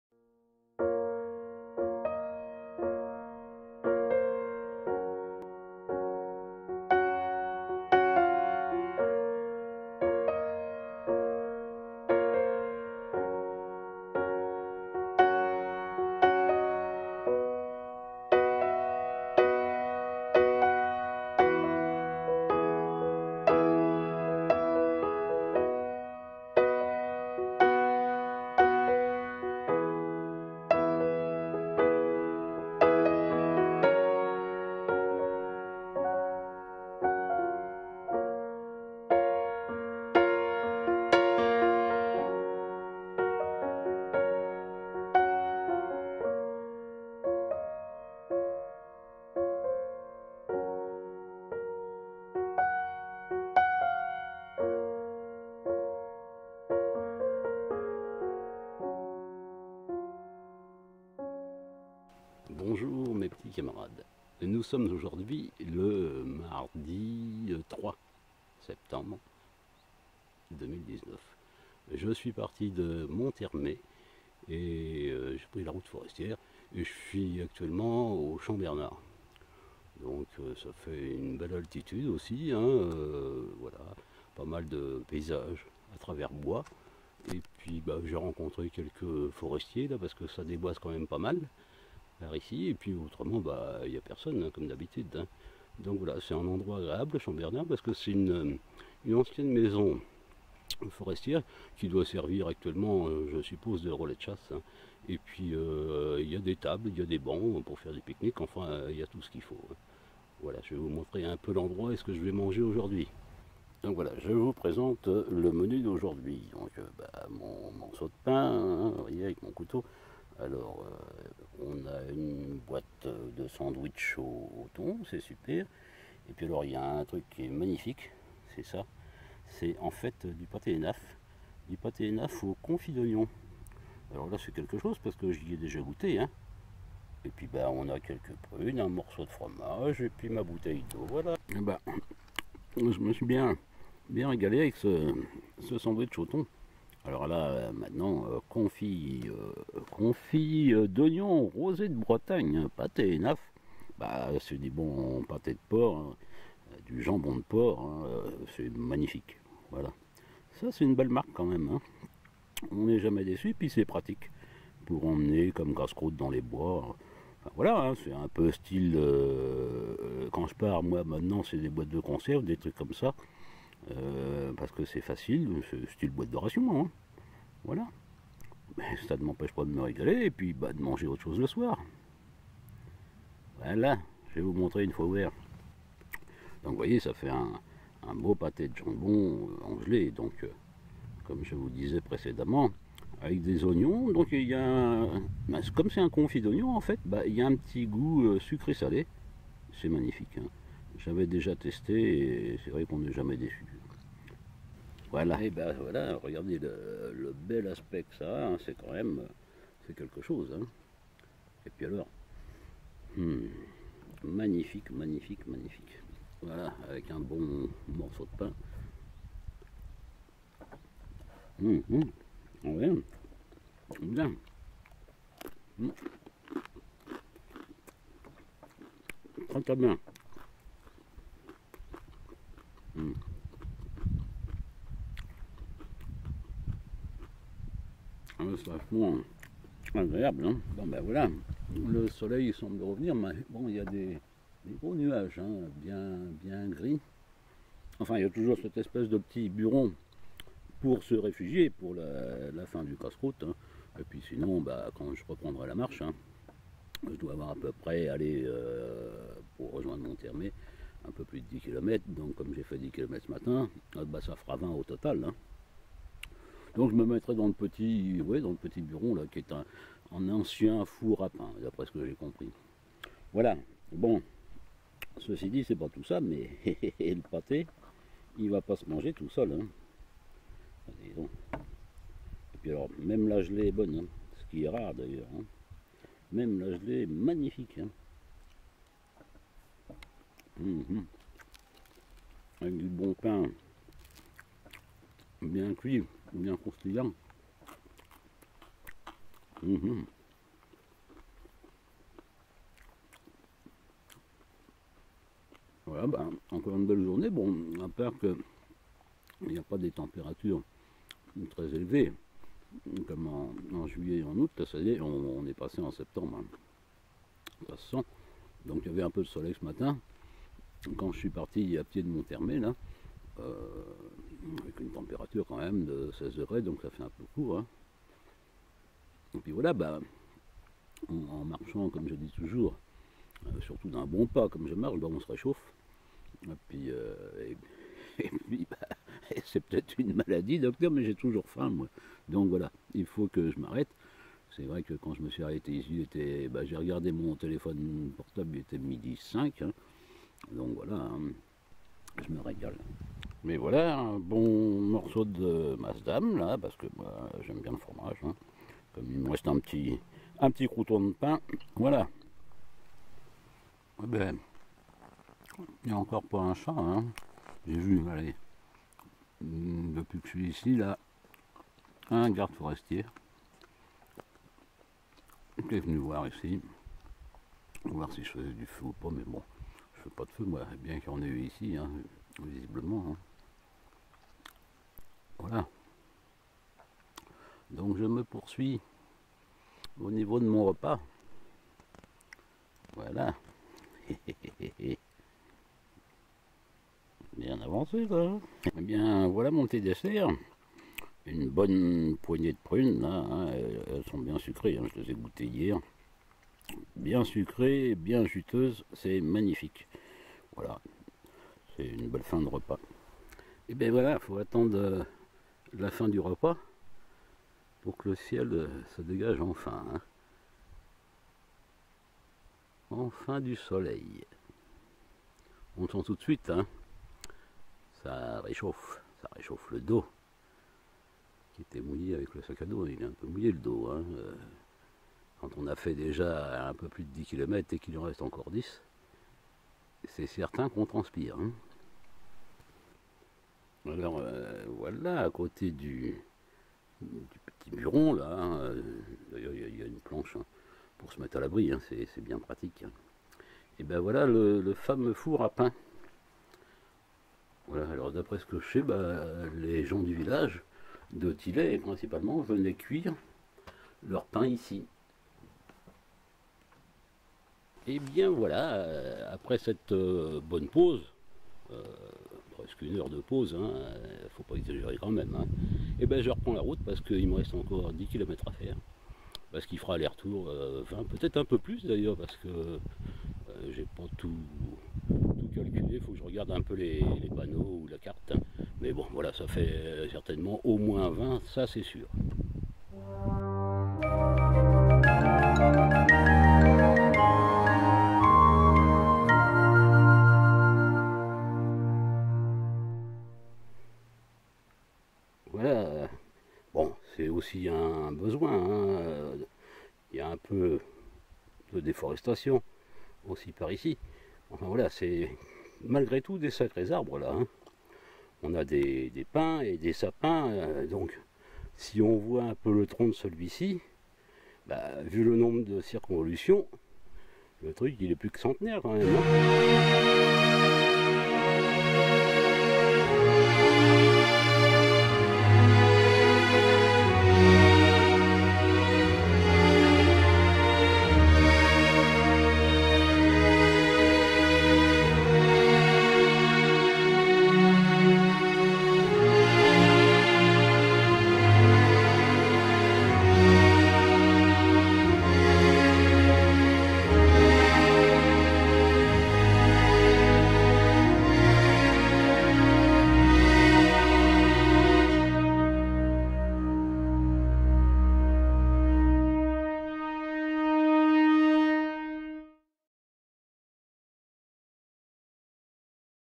Marche de 20 km et casse croûte au Champ Bernard
Après une marche de 10 kilomètres, au départ de Monthermé jusqu'au Champ Bernard par une route forestière, petite pause casse-croûte sur le Site de cette maison forestière qui comporte également un abri et un ancien four à pain. Après ce moment bien agréable,malgré un temps couvert et une température relativement  modérée, retour vers mon point de départ... Ce qui fera un parcours de 20 kilomètres au total.